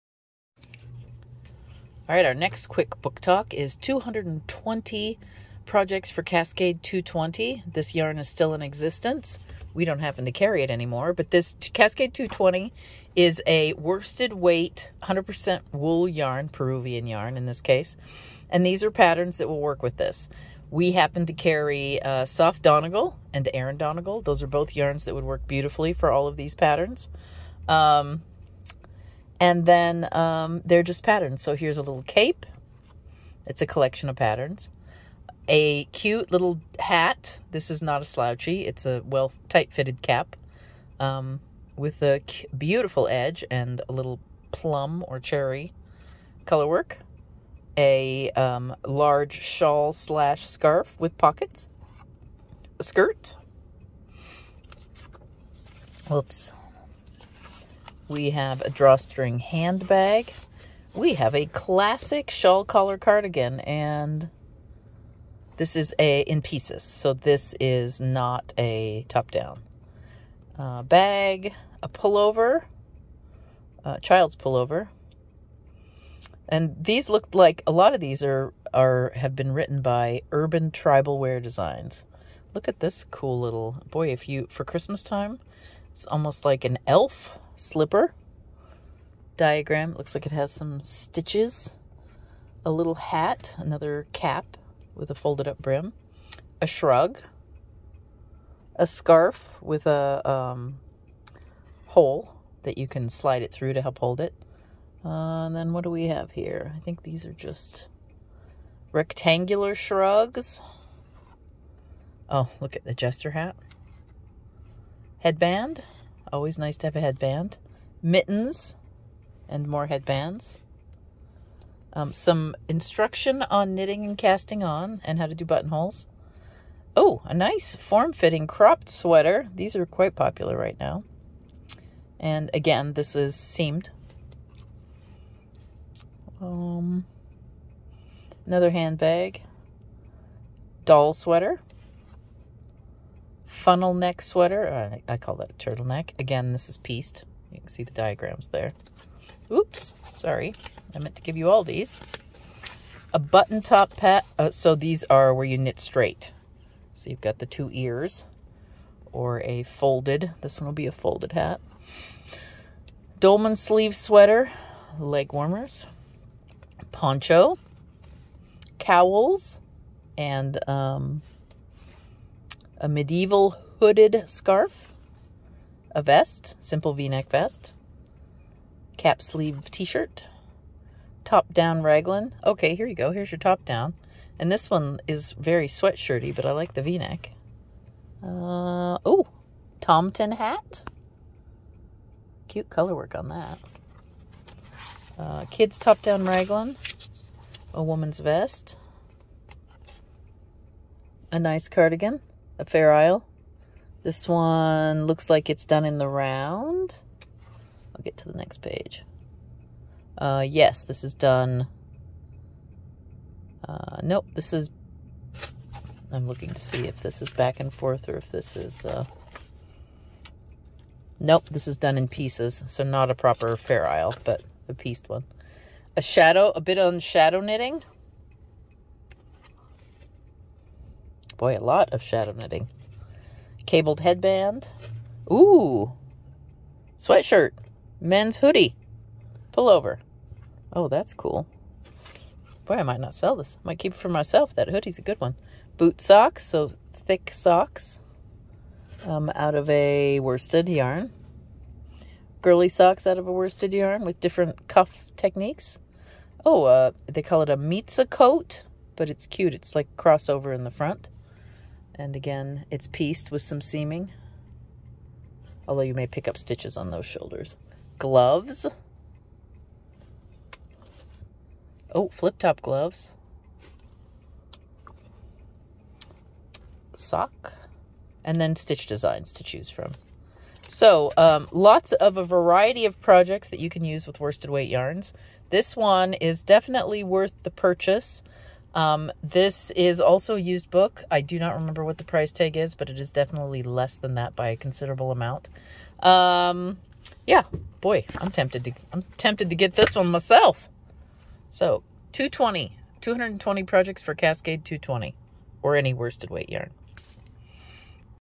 Quick Book Talk: 220 Projects for Cascade 220
Great staple set of patterns for all worsted weight yarns!